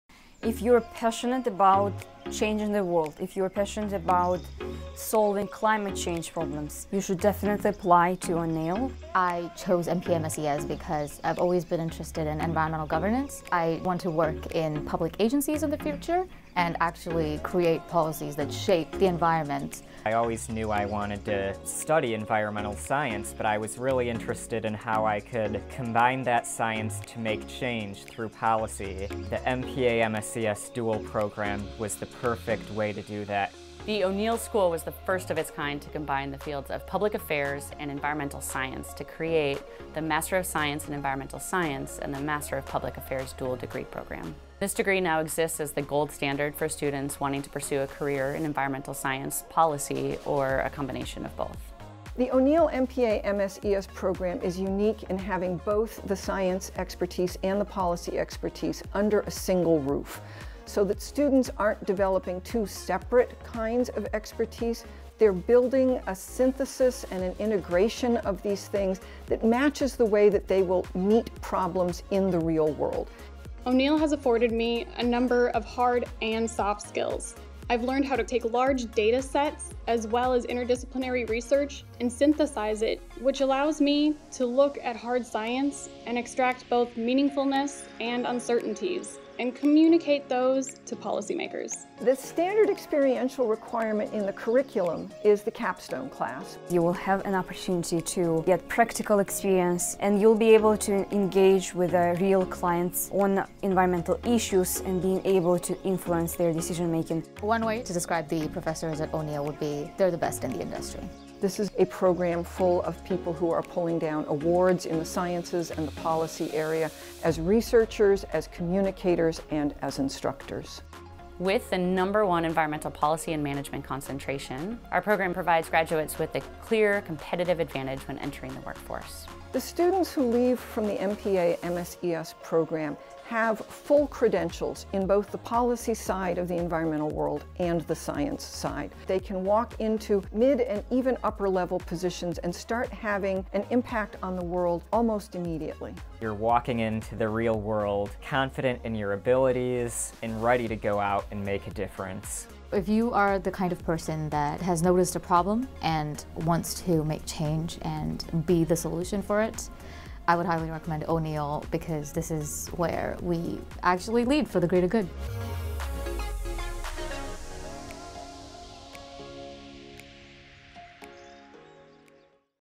Master of Public Affairs - M.S in Environmental Science at the O'Neill School (Indiana University)
Our signature dual degree program combines rigorous scientific training with one of the nation’s best masters programs in policy and management for a powerful and flexible combination of skills—positioning you at the crucial intersection of science, policy, and society.

Our MPA program is one of the top-ranked in the nation, and our environmental policy program is the nation’s highest-ranked program at any institution, public or private. You can earn both degrees in less time than it would take you to complete these degrees independently.